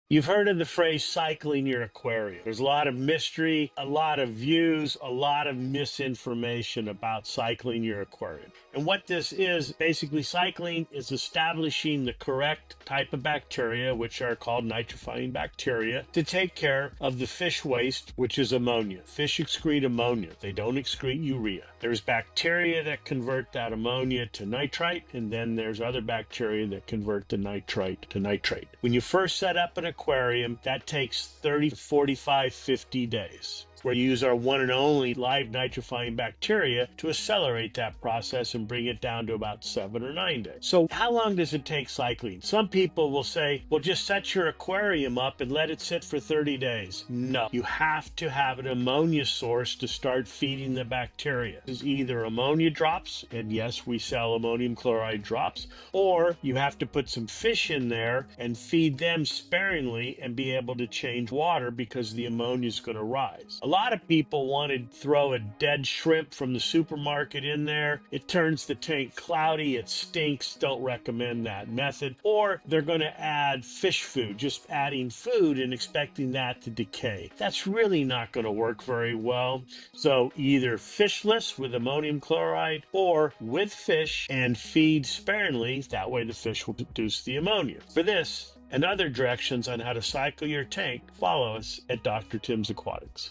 Faster Aquarium Cycle? Do This First
Cycling your aquarium means growing good bacteria to handle fish waste called ammonia.

These bacteria change ammonia to nitrite, then nitrite to nitrate.

Normally, this takes about 30 to 50 days, but live bacteria can speed it up to one week.

To cycle properly, you need a source of ammonia, either drops or a few fish fed lightly.

Follow for more expert tips on how to properly cycle your aquarium and keep your fish healthy.